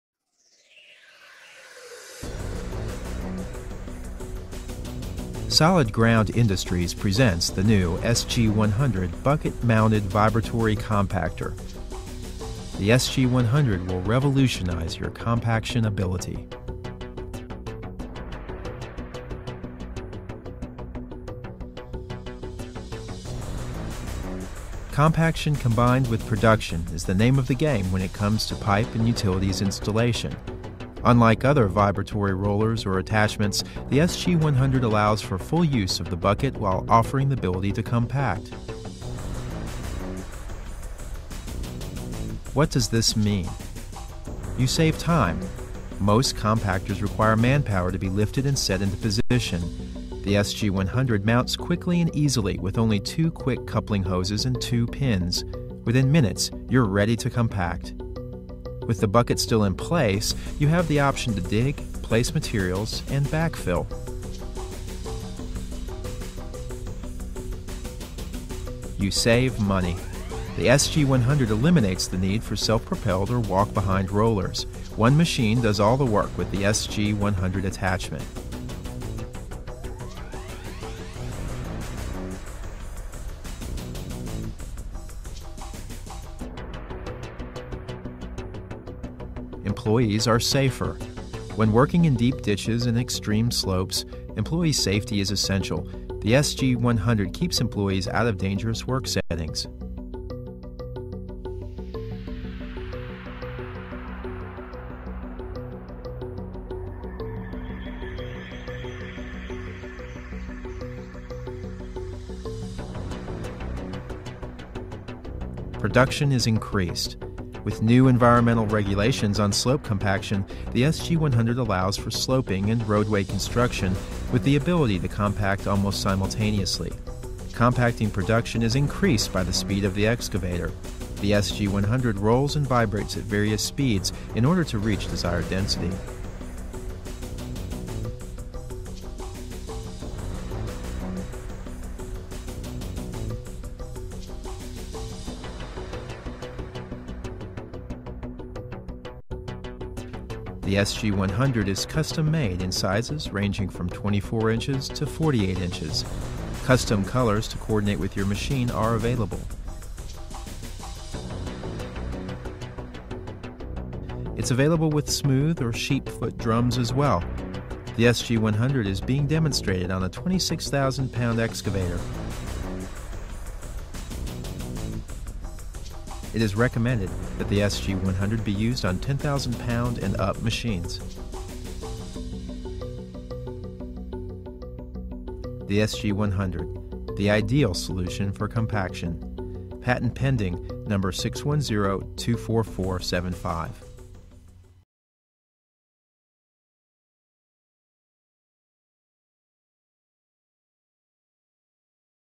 SG100 Soil Compaction Vibratory Compactor Attachment
SolidGroundIndustry.com launches the final product of our invention. This is a vibratory compactor that fits on the back of an excavator bucket and replaces man power and other machines. Efficiency at its' best.